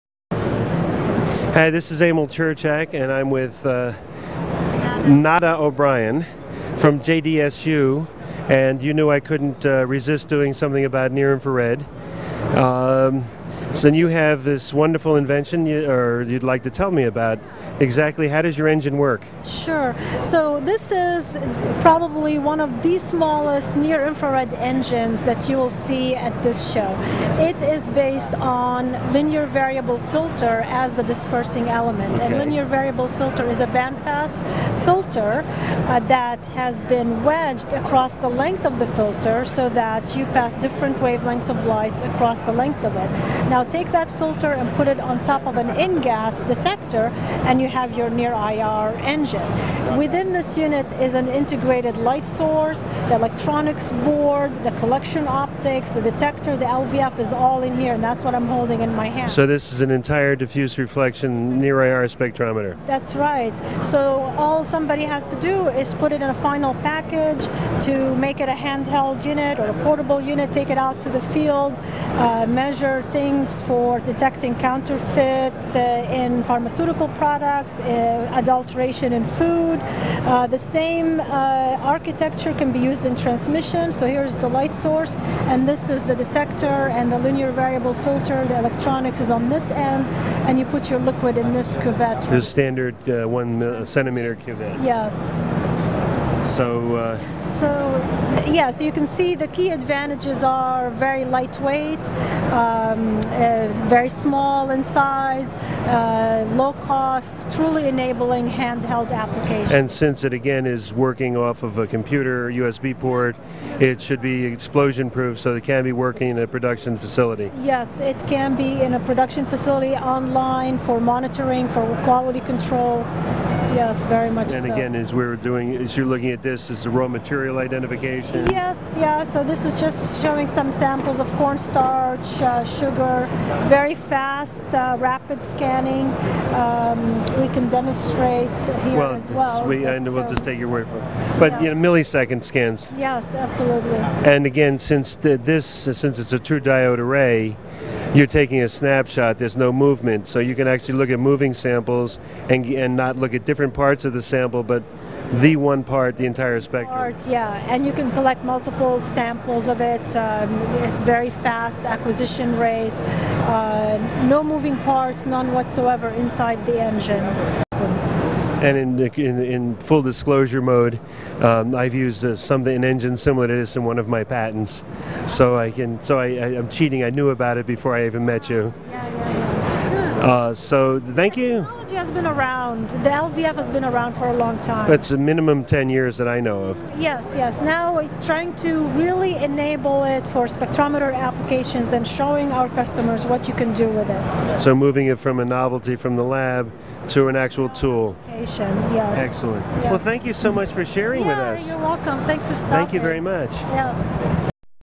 Pittcon Picks: The NIR Engine That Could for Pharma
Nada O'Brien of JDSU discusses the company's new fist-sized NIR engine and its many potential pharmaceutical applications, at Pittcon 2011 in Atlanta. The product was one of expert Emil Ciurczak's picks for truly innovative technologies shown at this year's show.